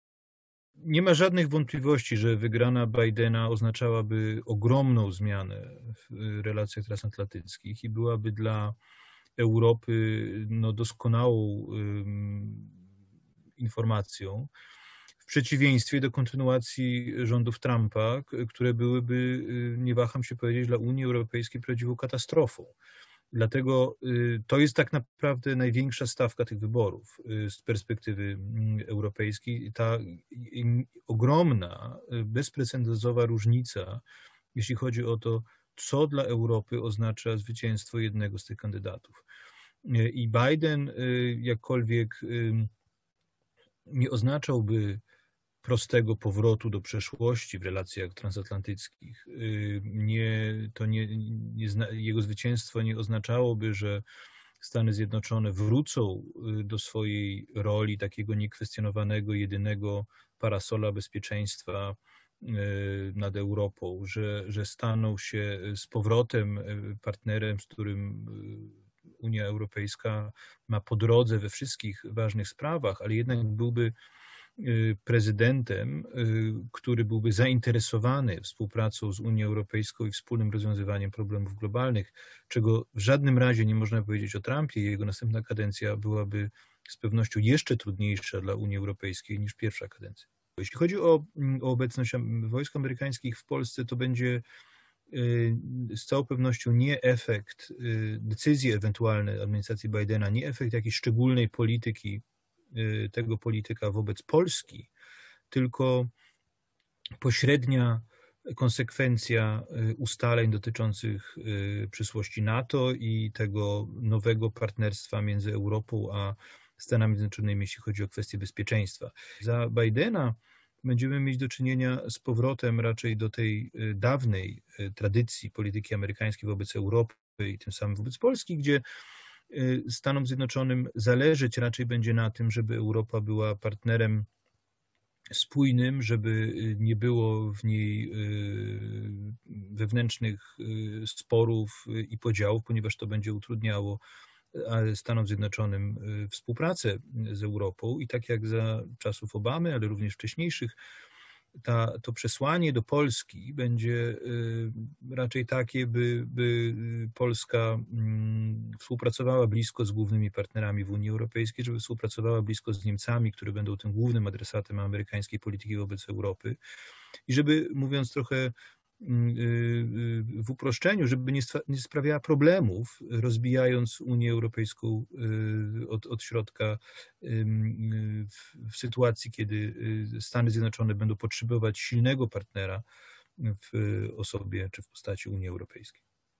Amerykańskie wybory na ostatniej prostej.
W środę 4 listopada możemy poznać wyniki wyborów za oceanem. Na razie sondaże wskazują na zwycięstwo demokraty Joe Bidena, nie jest ono jednak przesądzone. Kluczowe będą głosy w kilku stanach, w tym Pensylwanii czy na Florydzie. Eksperci wskazują, że głosy w sondażach na rzecz reelekcji obecnego prezydenta Donalda Trumpa mogą być niedoszacowane, bo część pytanych nie przyznaje się, że będzie na niego głosować. Republikanin może też kwestionować wyniki na drodze sądowej, co opóźni rozstrzygnięcie. Prezydentura Bidena będzie dla UE lepsza, choć też niepozbawiona wyzwań.